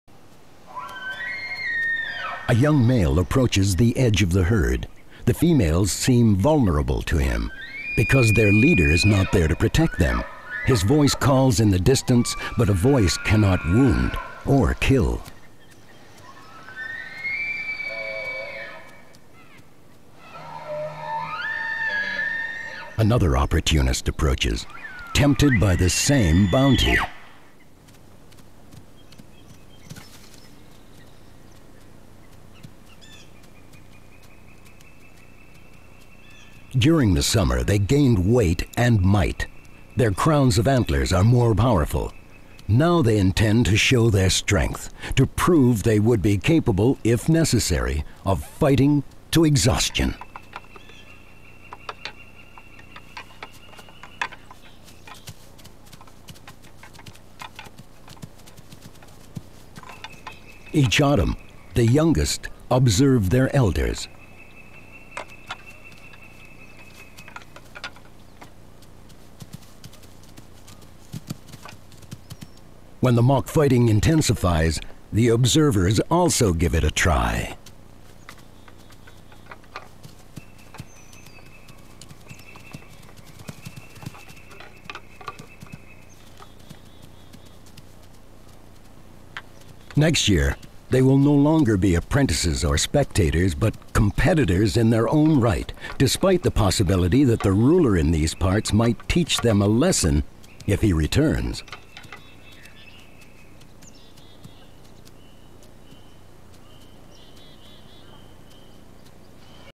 Rocky Mountain Wildlife
Several young bulls spar with each other while the herd of females looks on.